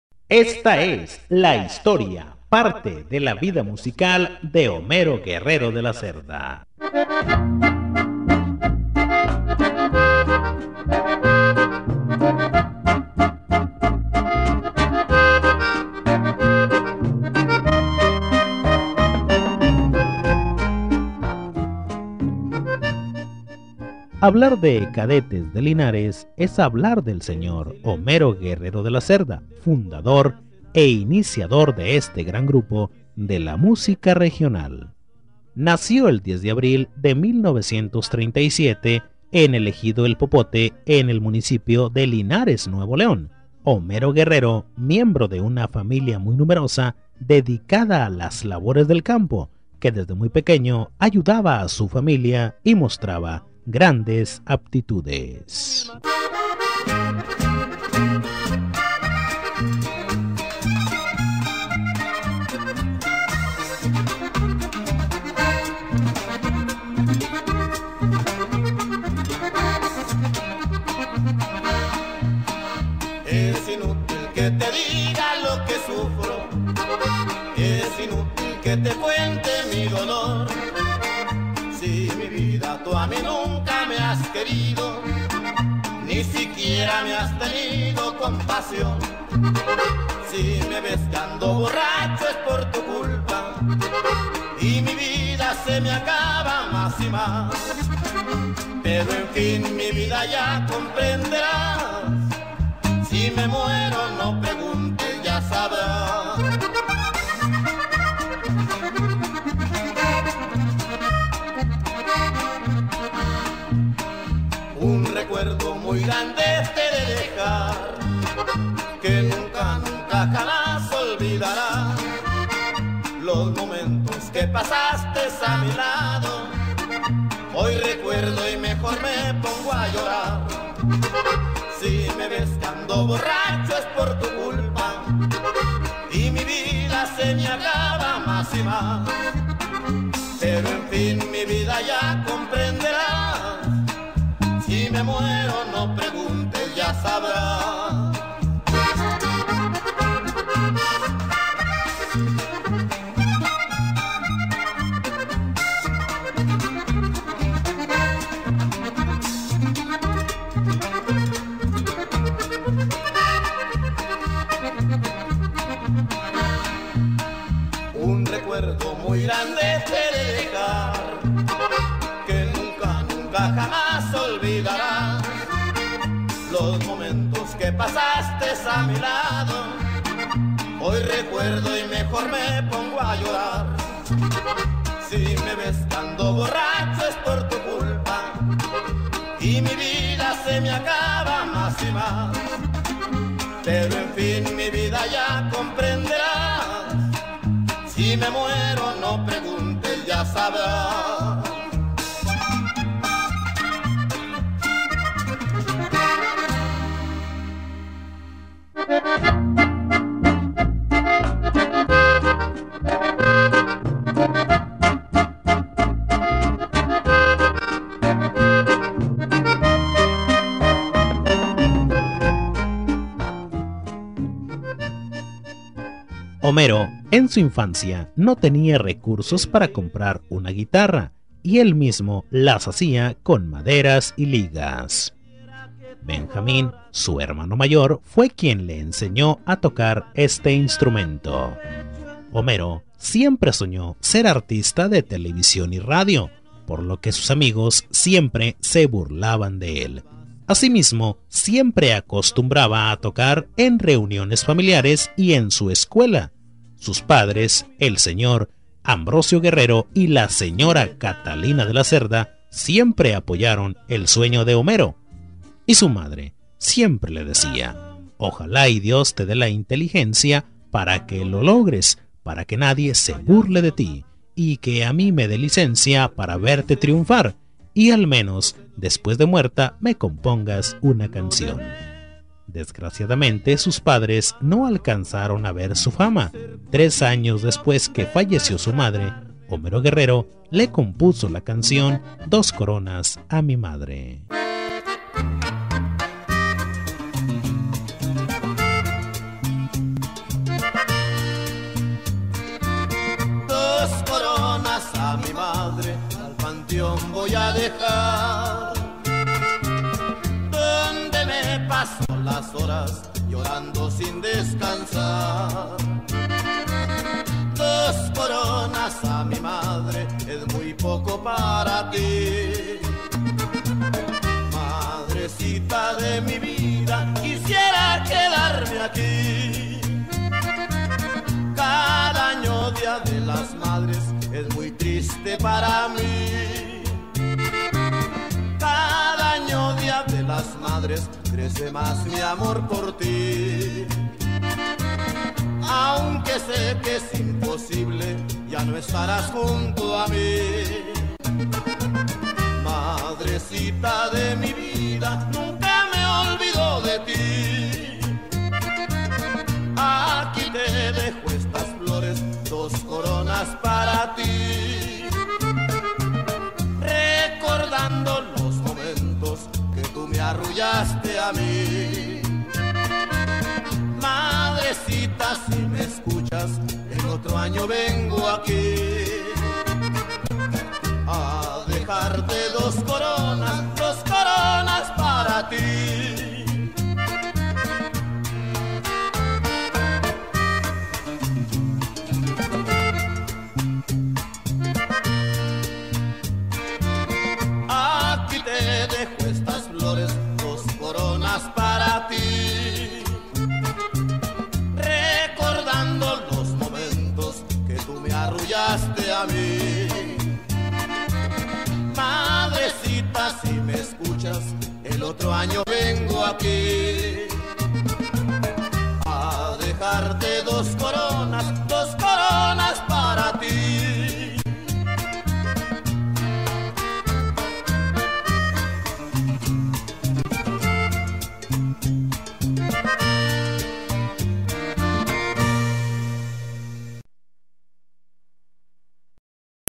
HOMERO GUERRERO EL AUTENTICO CADETE LA BIOGRAFIA P1 D 6
RECORDANDO  PARTE DE LA VIDA MUSICAL DE HOMERO GUERRERO EN UN ANIVERSARIO MAS DE SU SENSIBLE FALLECIMIENTO   HISTORIA NARRADA POR ISAAC ZAPATA LOCUTOR DE XEWQ DE MONCLOVA COAH   TOMADA DE LA REVISTA SINTONIA MUSICAL